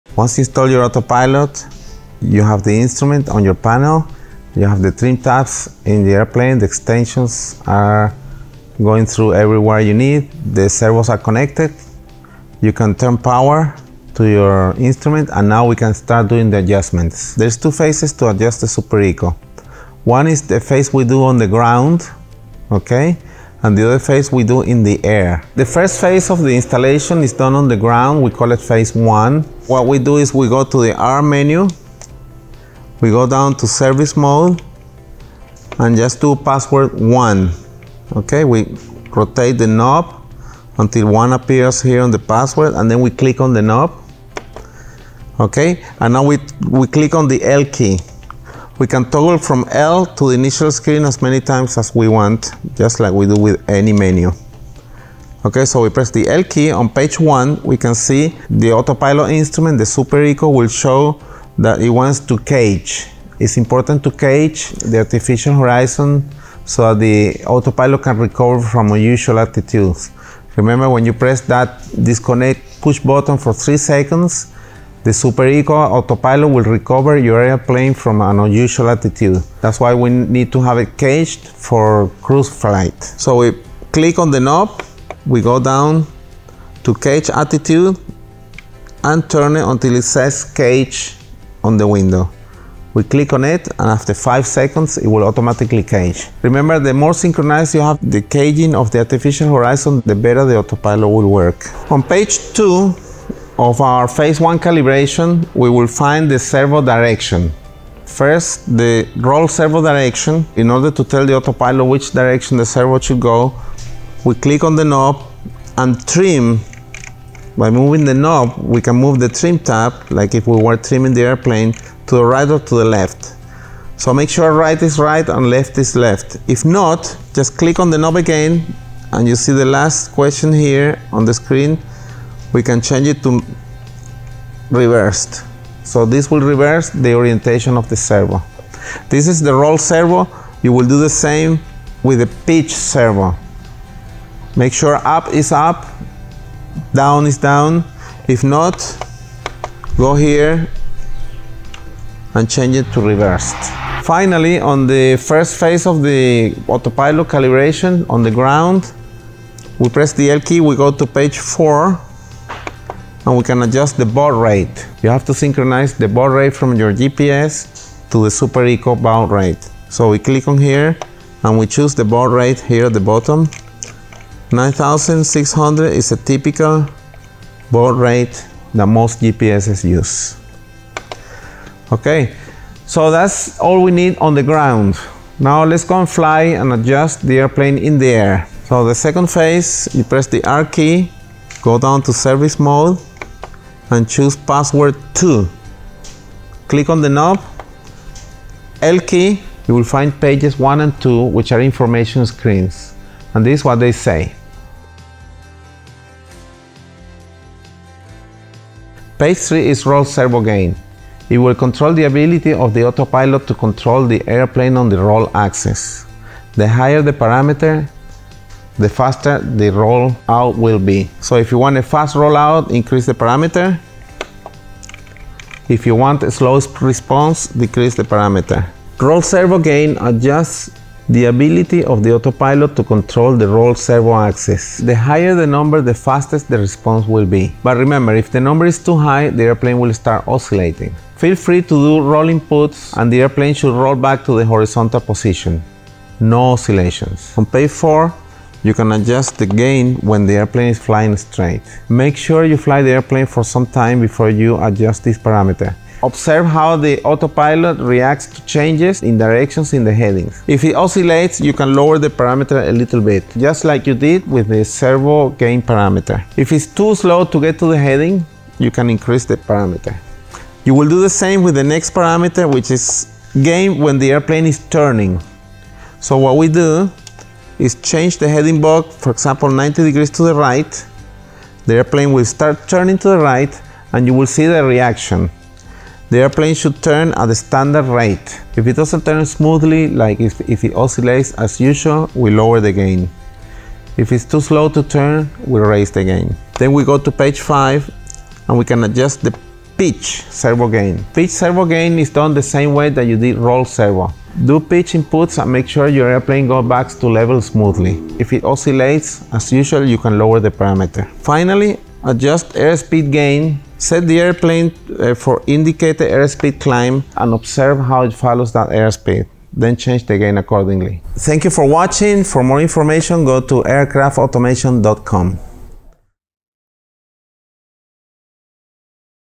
How to Adjust and Calibrate the Aircraft Automation SuperECO Autopilot
You can now control the pitch, roll, and yaw of your light-sport or experimental aircraft with a carbon-fiber trim tab autopilot that weighs less than 2.5 lbs. It's time to redefine the way you fly and turn your cockpit automation dreams into reality without compromising weight, safety, and reliability.

Contact Support
Mon-Fri 9am-5pm Eastern